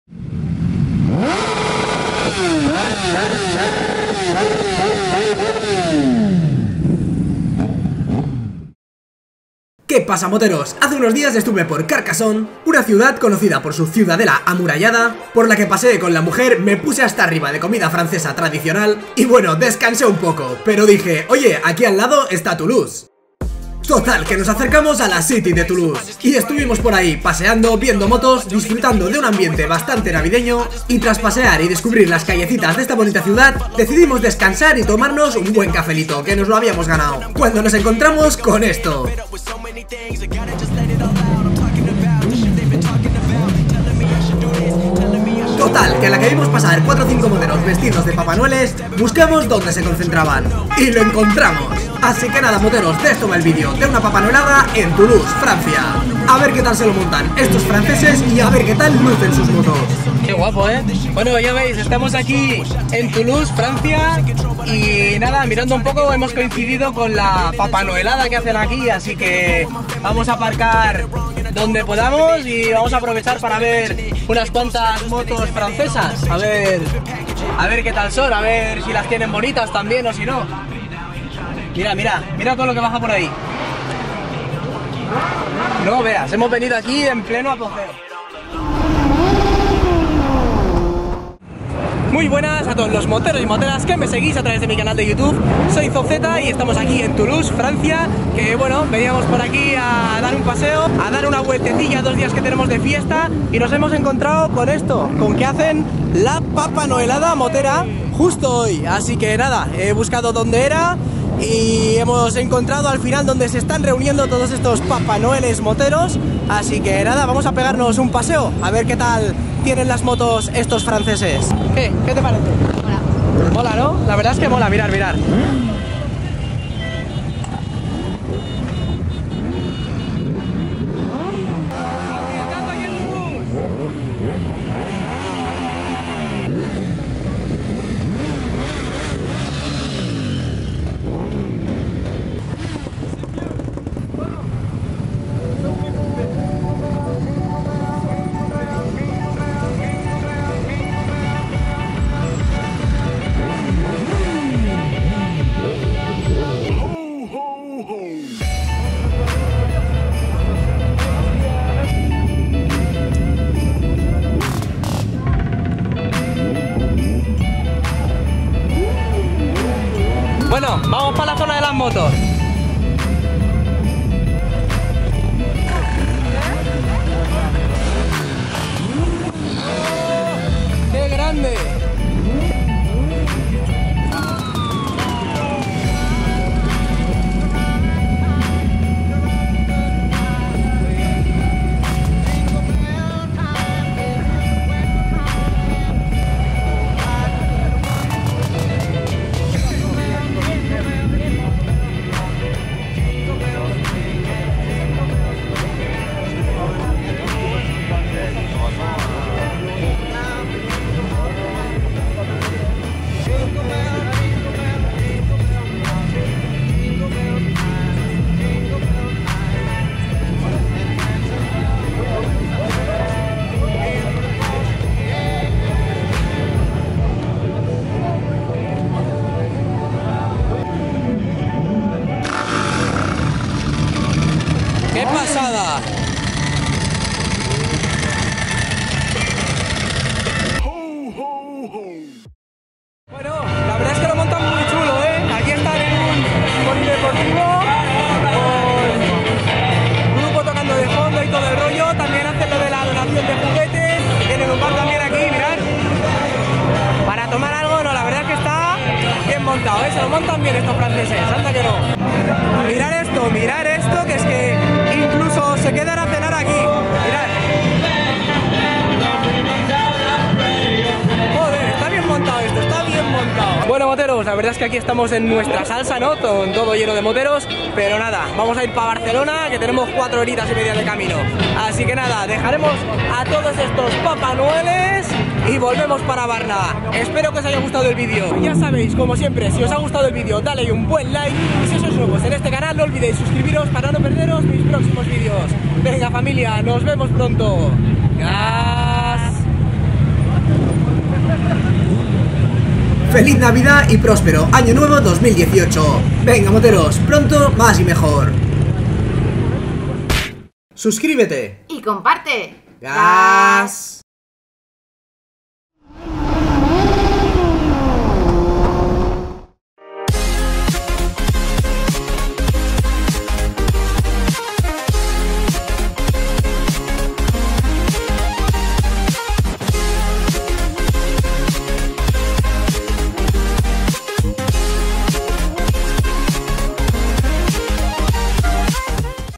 PAPA NOEL RIDERS MOTEROS EN FRANCIA !!! HOU HOU HOU !!!
Papas Noeles en Moto!!! Os invito a ver conmigo una quedada de Papa Noel Riders Franceses que ví hace unos días en mi paso por Toulouse!!!

Espero que el vídeo os guste Moter@s!!! Feliz Navidad!!! ;) :P

Pronto más y mejor!!! jeje

Gracias por los Likes & Subscribes!!!

GasSsSsSsss!!!